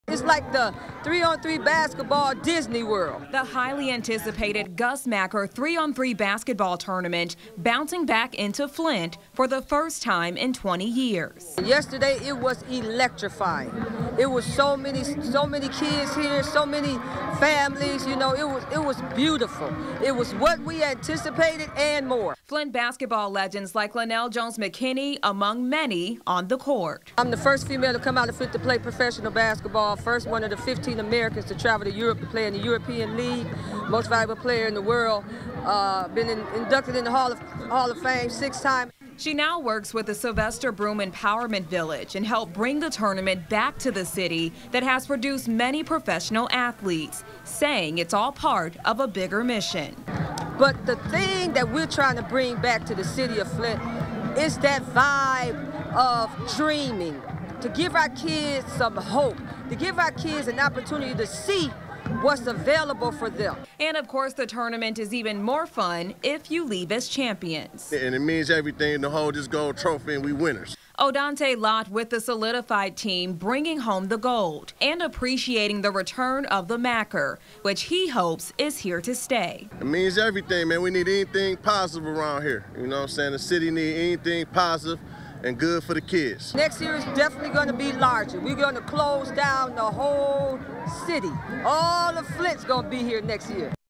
Gus Macker Tournament returns to Flint after 20 years
Basketball lovers united in Flint for the Gus Macker Tournament.